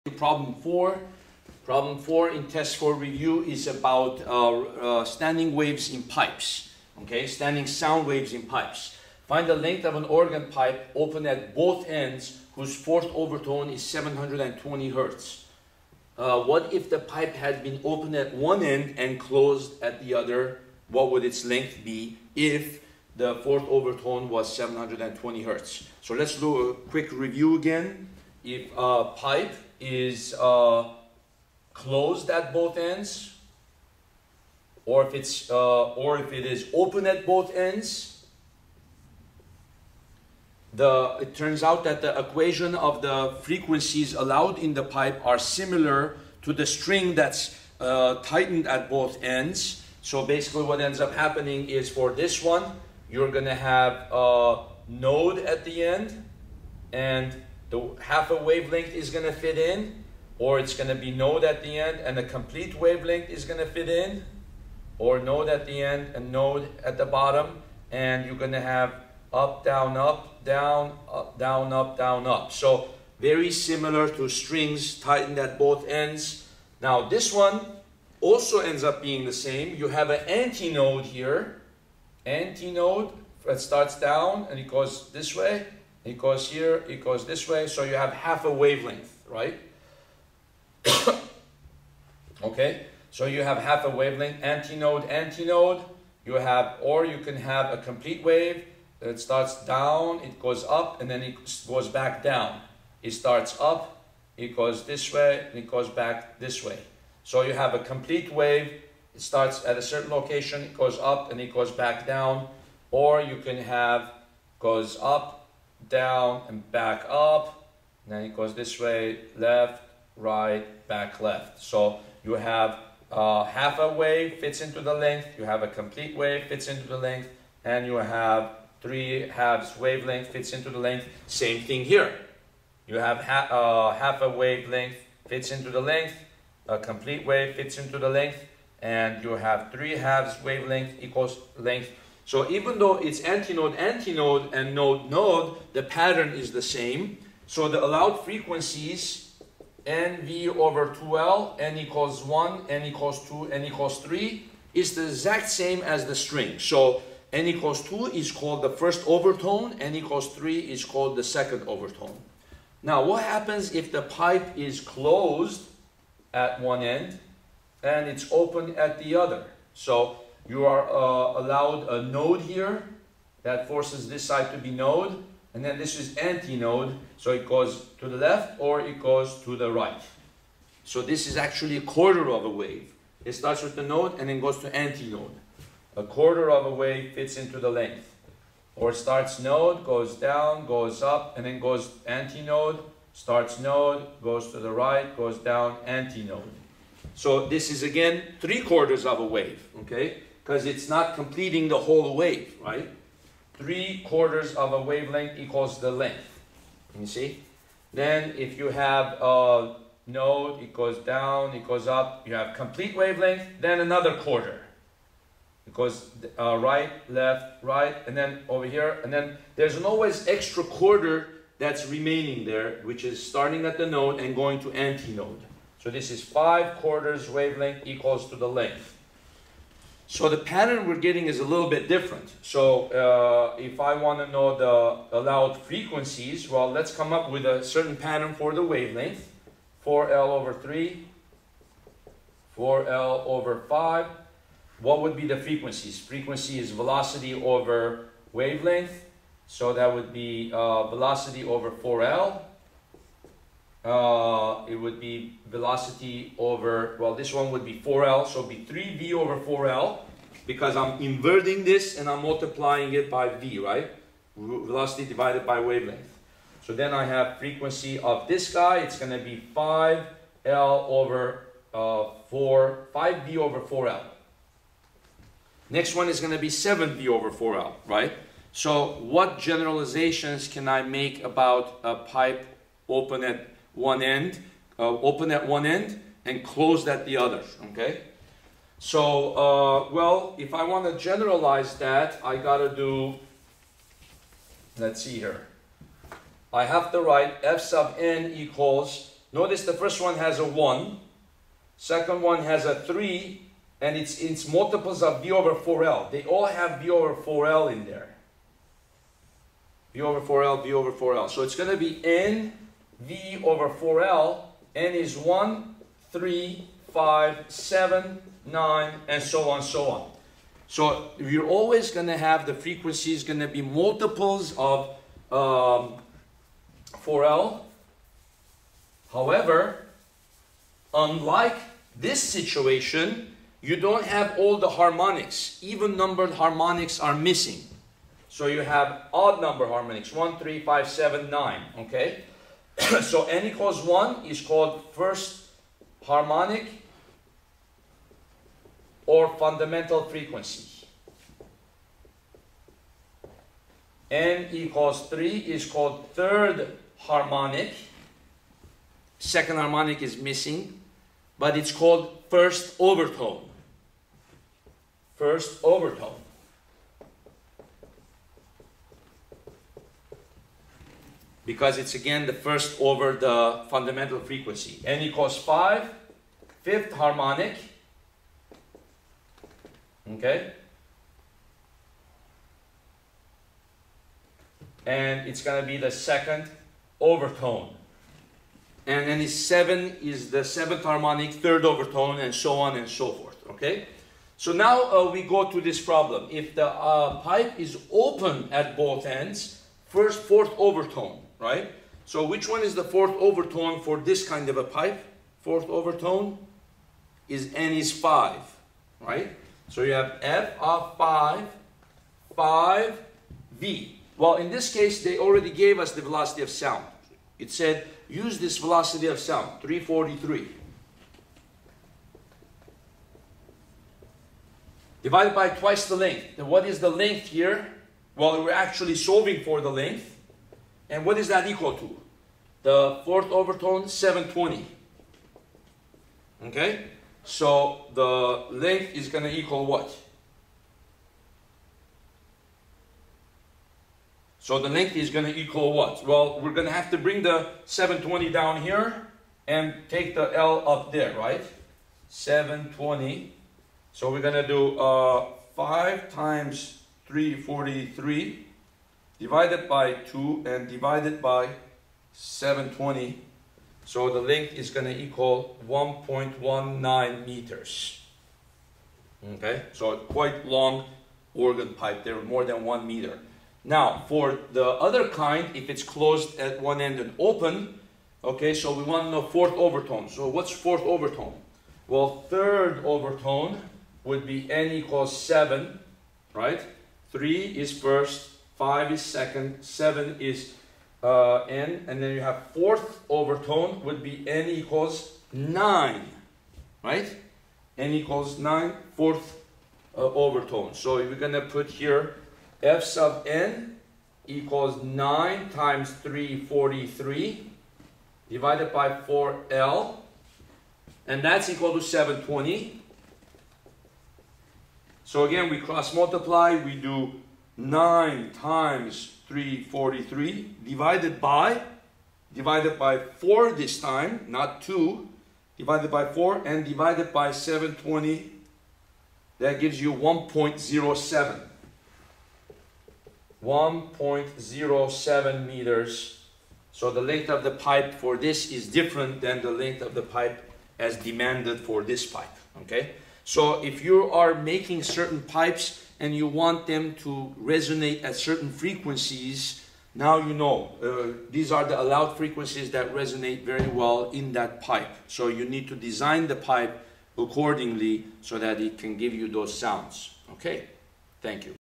PhysicsBootcampTest4ReviewP4(Sound Resonance).avi
This video is part of the Physics Bootcamp series. It covers the fourth problem of the fourth review test for this course.
Find the length of an organ pipe open at both ends whose 4th overtone is 720 Hz.
What if the pipe had been open at one end and closed at the other?  Take vs = 343 m/s